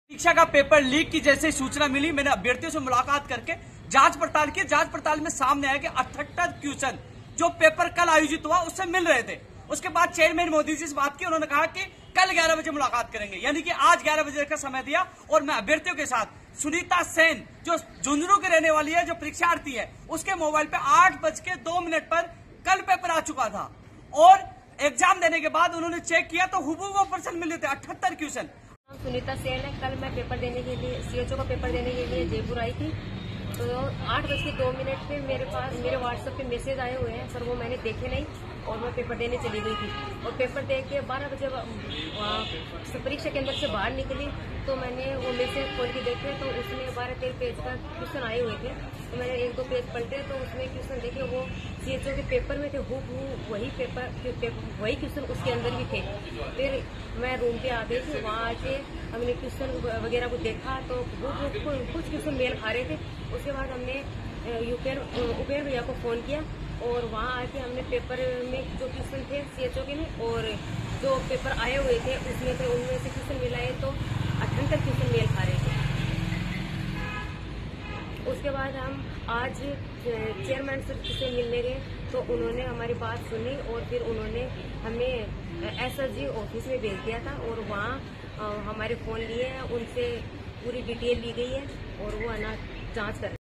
Rajasthan cho bharti pariksha paper leak news today | cho पेपर लीक मामले में उपेन यादव का बयान
Rajasthan cho bharti pariksha paper leak latest news upen yadav cho pariksha